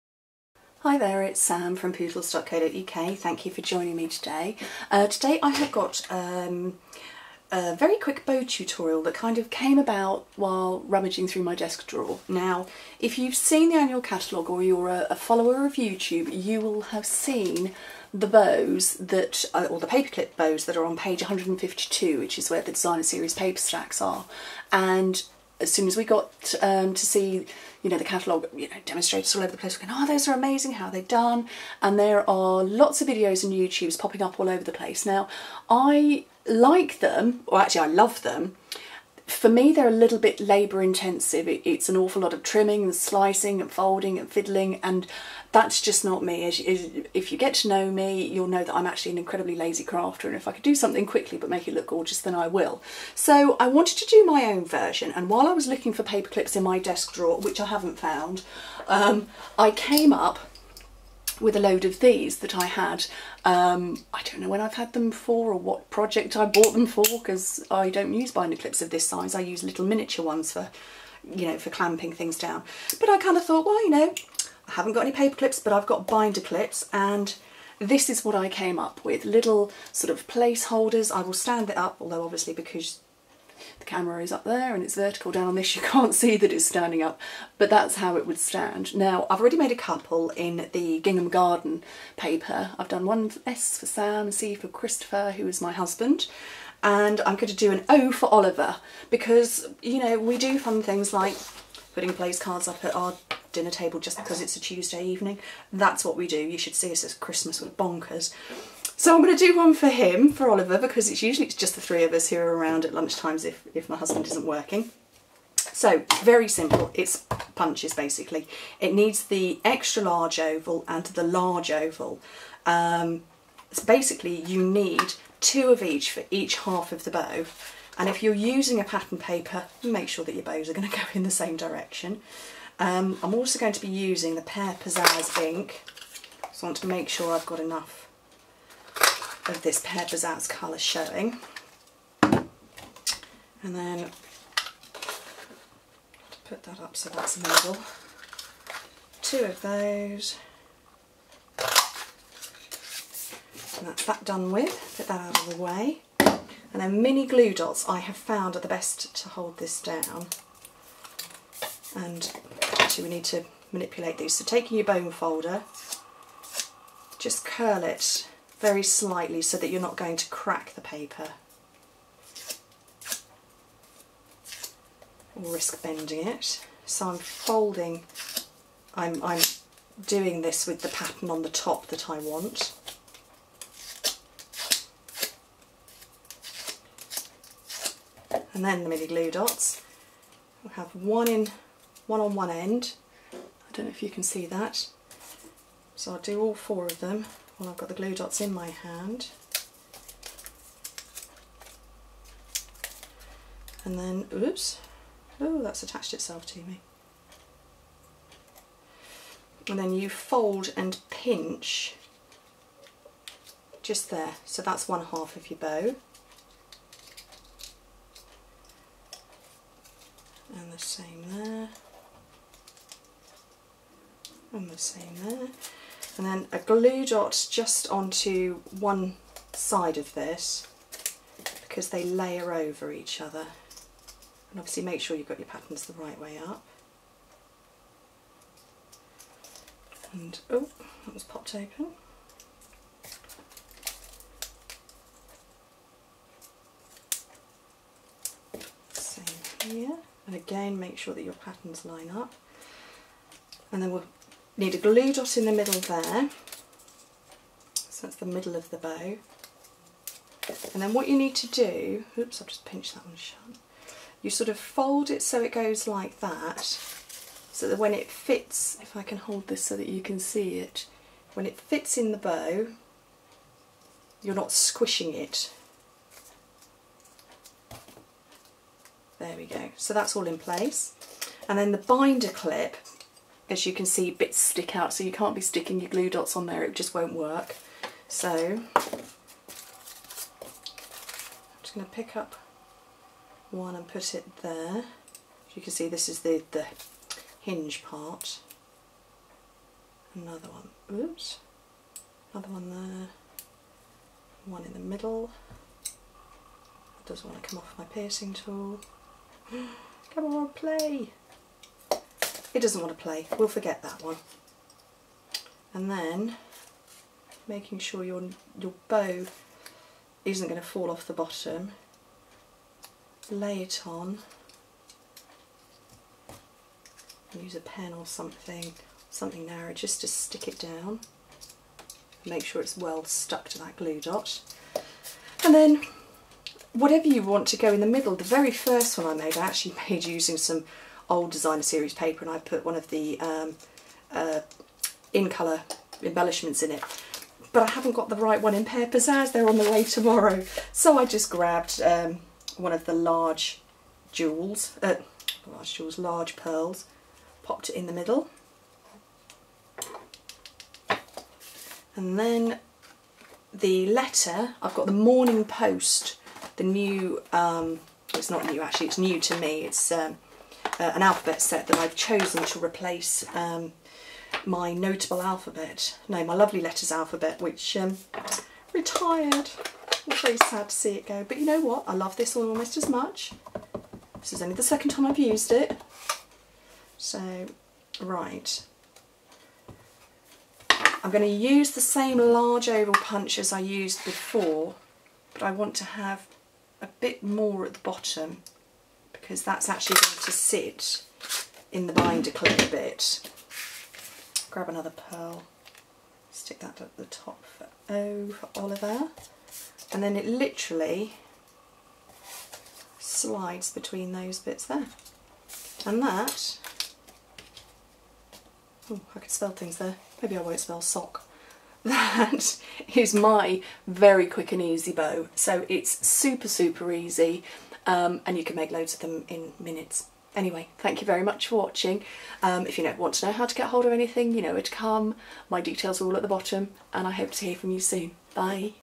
Stampin Up Paper Bow Punch Tutorial
Stampin' Up! UK Demonstrator Sam Donald shows her quick and easy punch bows!